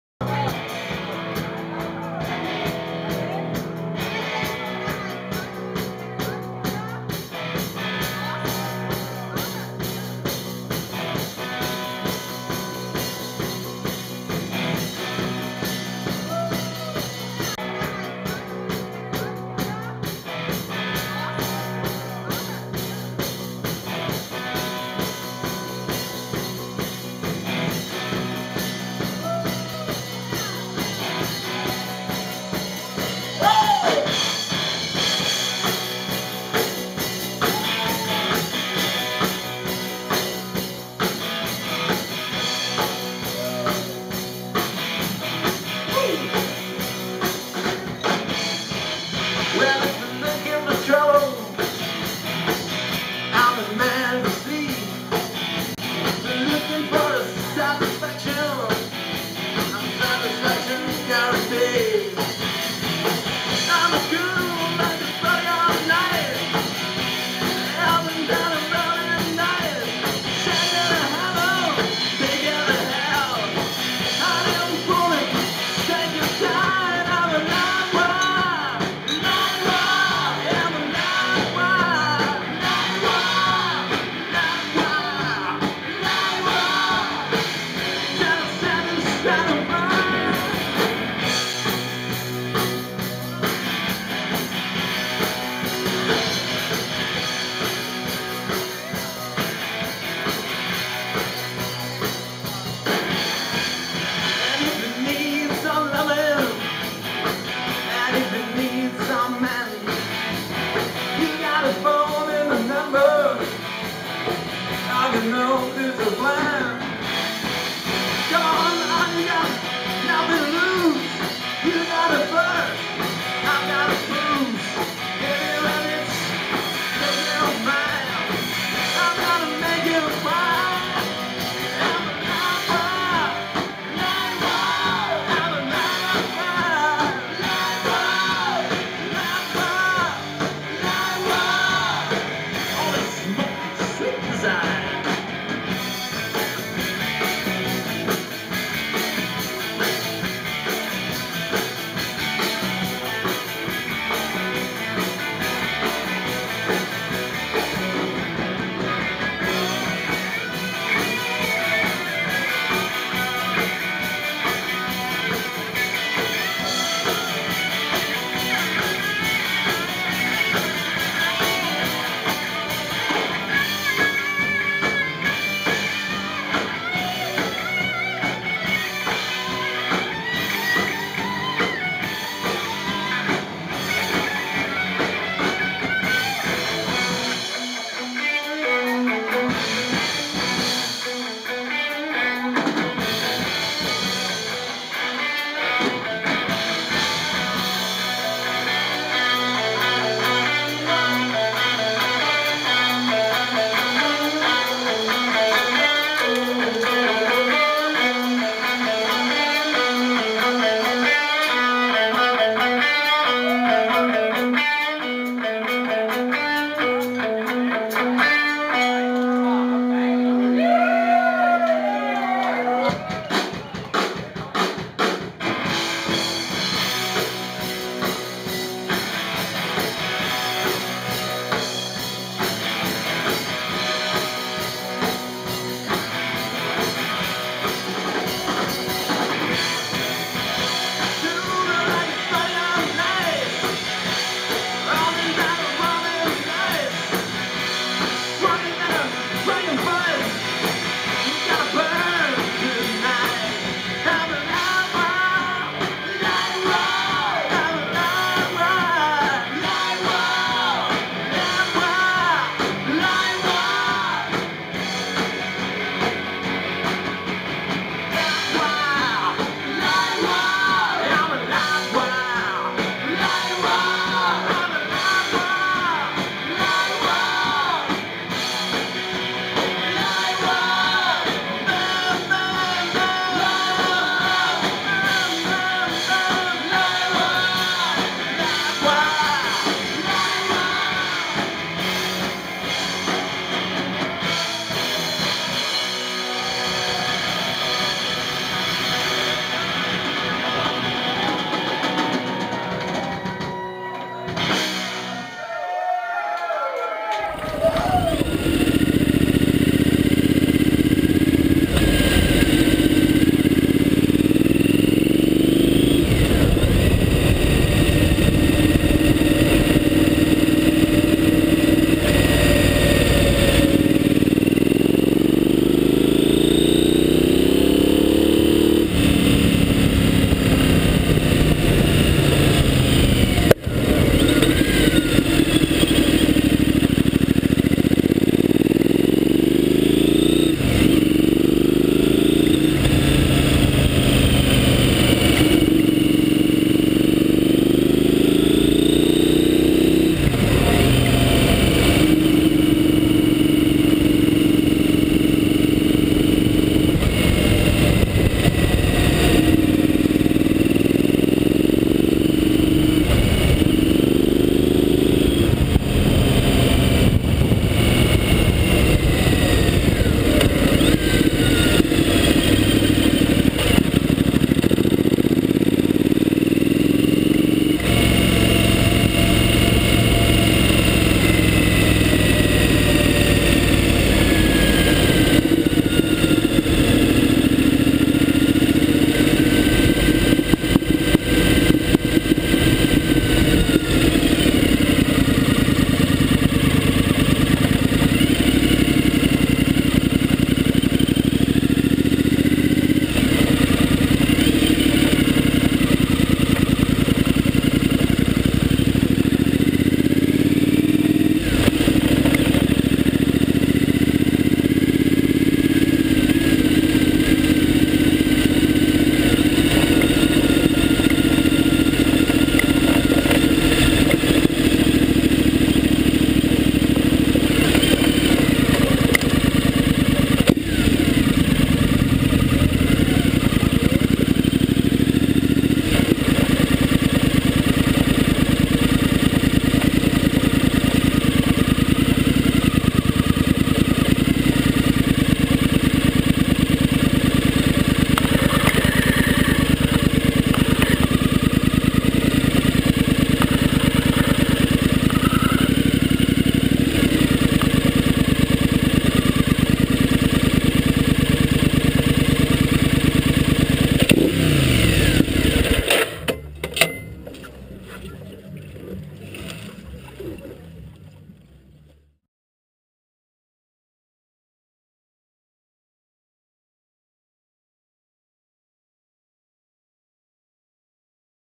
Sagatreffet 2012
Saga treffet, flott MC treff.
Bilde i bilde er turen min til treffet, og avslutter når jeg parkerer inne på Sagatreffet.